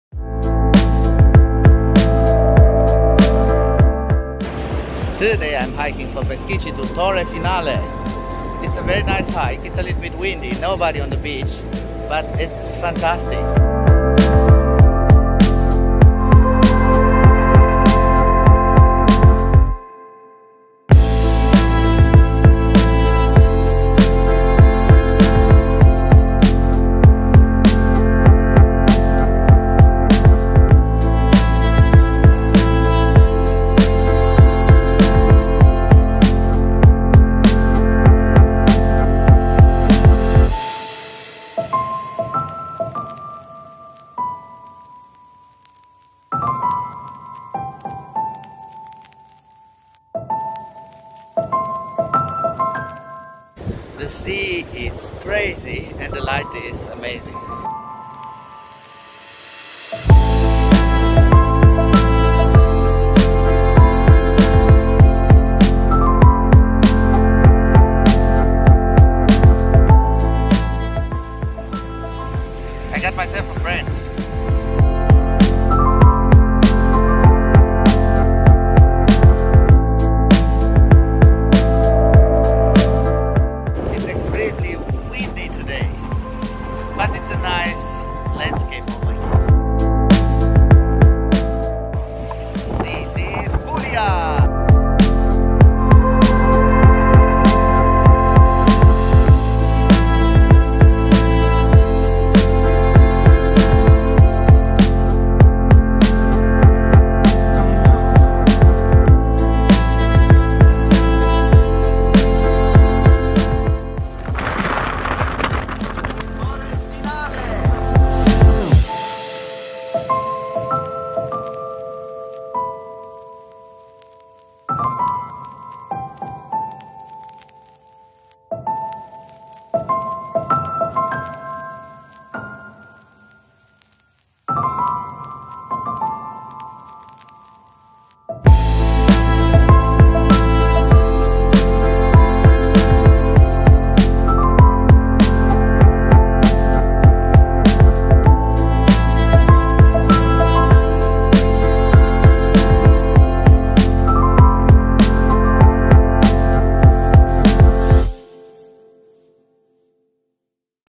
GARGANO - HIKING from PESCHICI 🏃‍♂️ to VIESTE with my DRONE & a DOG 🐶
GuideAngelo MicroVlog Daily Vlog Episode 115 SEASON PUGLIA 🇮🇹 GARGANO - lHIKING from PESCHICI 🏃‍♂️ to VIESTE with my DRONE & a DOG 🐶 by Guide Angelo